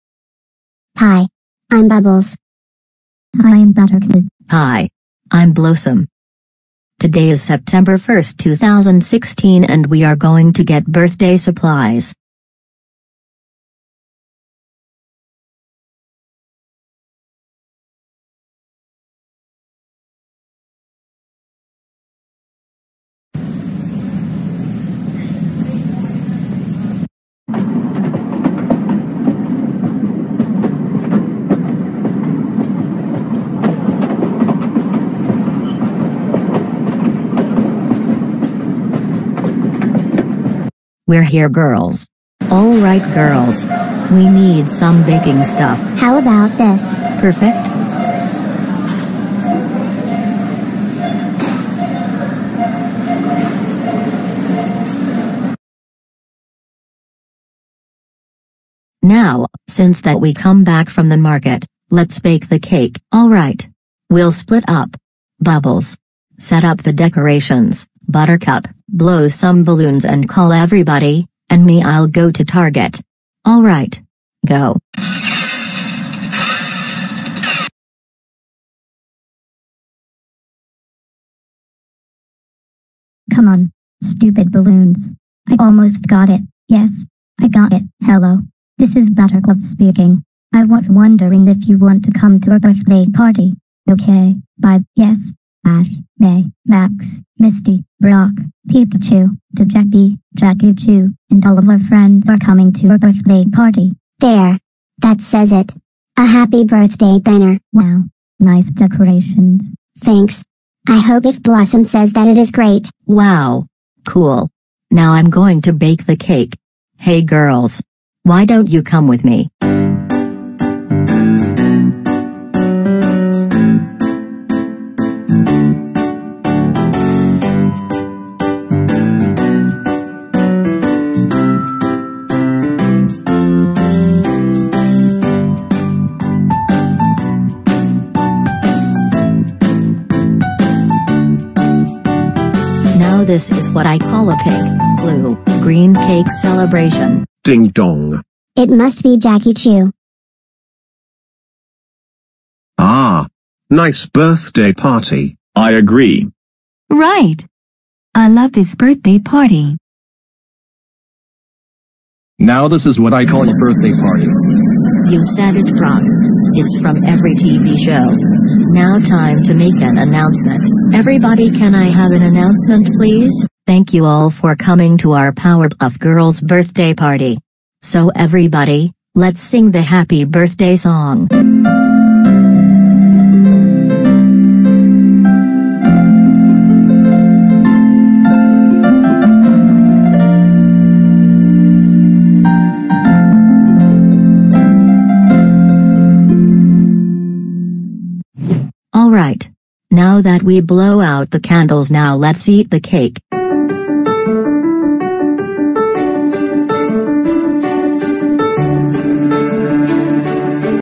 (REUPLOAD) Goanimate The Powerpuff Girls Birthday
Long before Go!Animate fans were aware that the site used to have a Powerpuff Girls theme thanks to its revival and inclusion on Wrapper Offline, there was this.